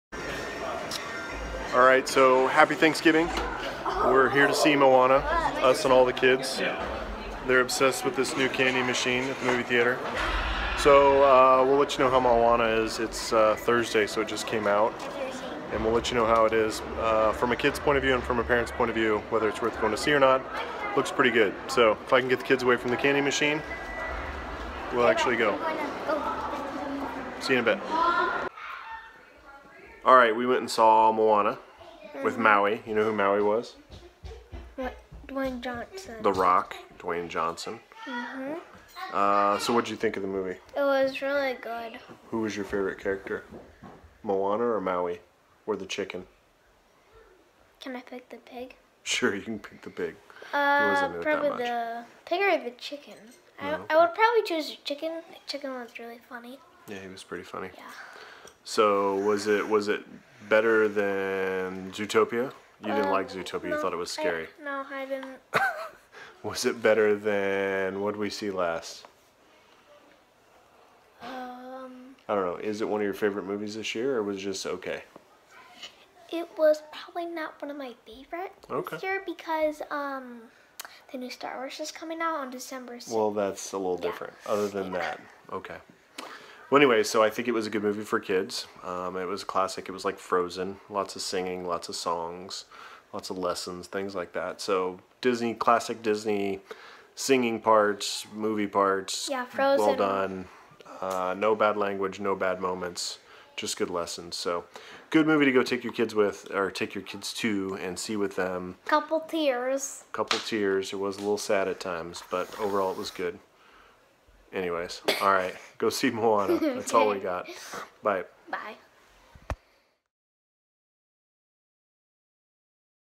Movies for Kids - Moana Movie Review
We took everyone to see Moana tonight.  Check out this video for a review by kids and parents.